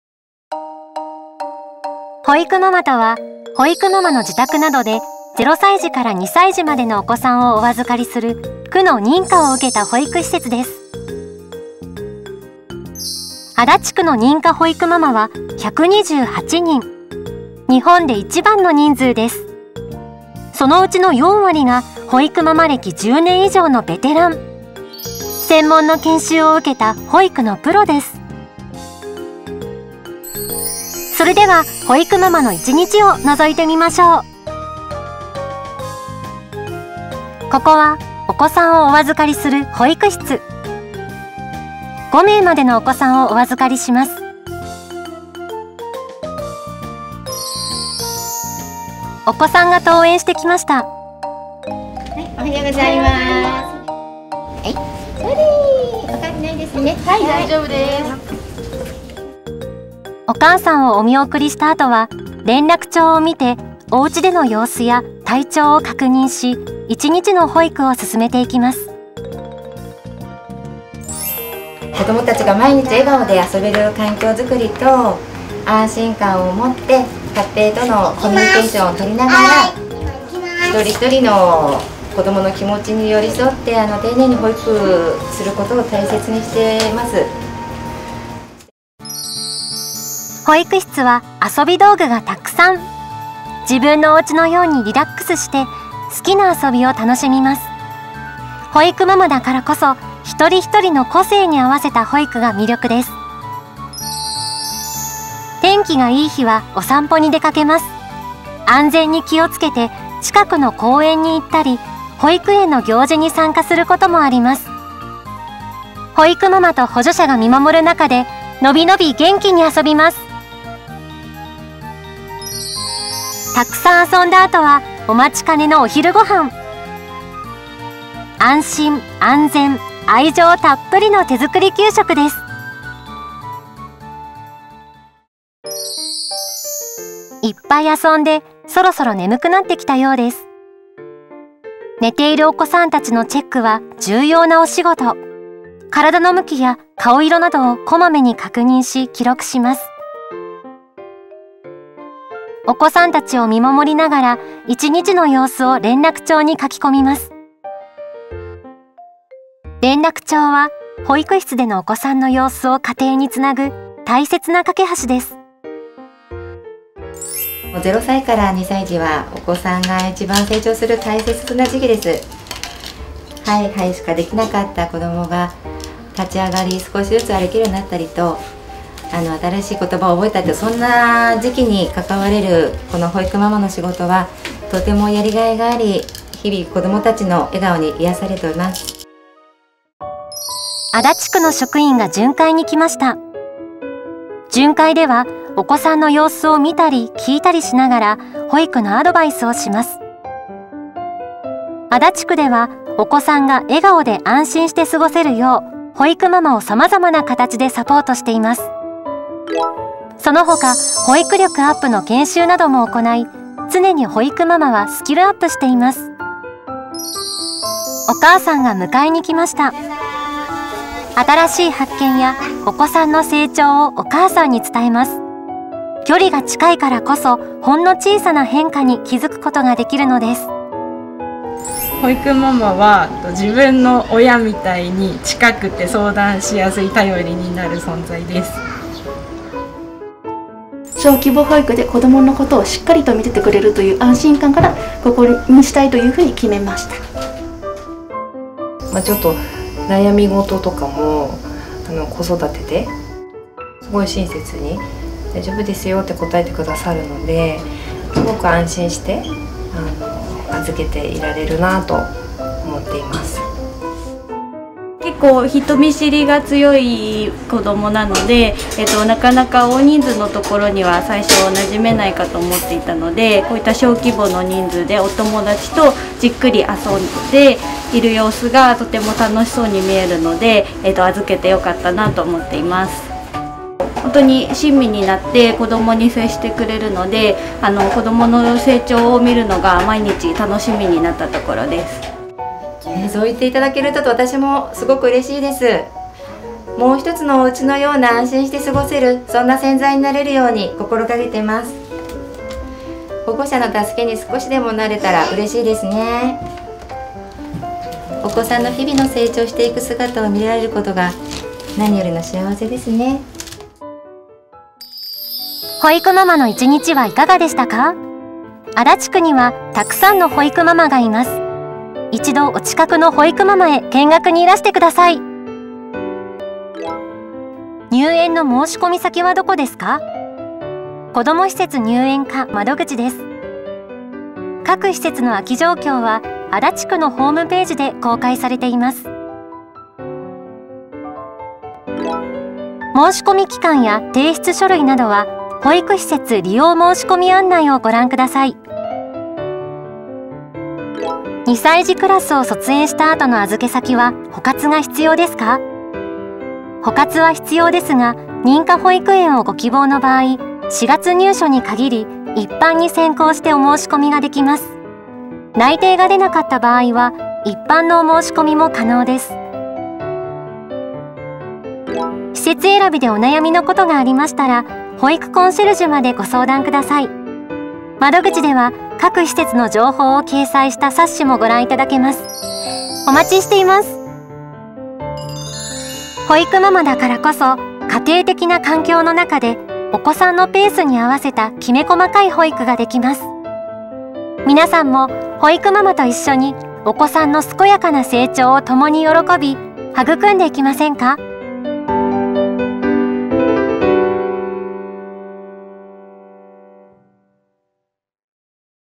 足立区保育ママの１日をのぞいてみよう！
保育ママとは、保育ママの自宅などで０歳児から２歳児までのお子さんをお預かりする保育施設のことで、区内には約９３人（令和６年４月時点）の保育ママがいます。
この動画では、保育ママ・利用者インタビューを交え、保育ママの一日を紹介しておりますので、是非ご覧ください！